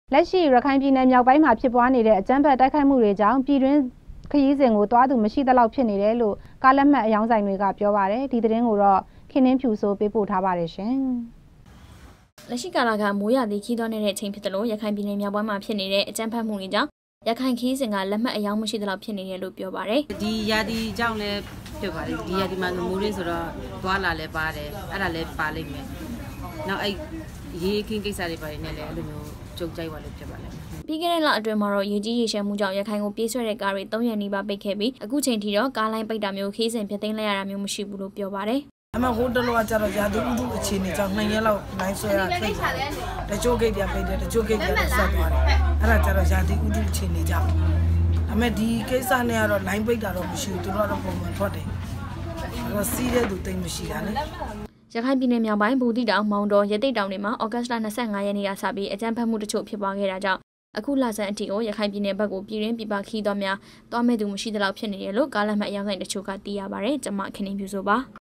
DVB - ရခုိင္ျပည္မွာအၾကမ္းဖက္မႈေတြျဖစ္ပြားေနလုိ႔ ျပည္တြင္းခရီးစဥ္သြားသူမရွိသေလာက္ျဖစ္
DVB TV - လက္ရွိ ရခိုုင္ျပည္နယ္ေျမာက္ပိုုင္းမွာျဖစ္ေနတဲ့ အၾကမ္းဖက္တိုုက္ခိုုက္မႈေတြေၾကာ င့္ျပည္တြင္းခရီးစဥ္ကို သြားသူမရွိသေလာက္ျဖစ္ေနတယ္လို့ ကားလတ္မွတ္ အေရာင္းဆိုင္ေတြေျပာပါတယ္။ခင္နွင္းျဖဴစိုးေပးပို့ထားတာပါ။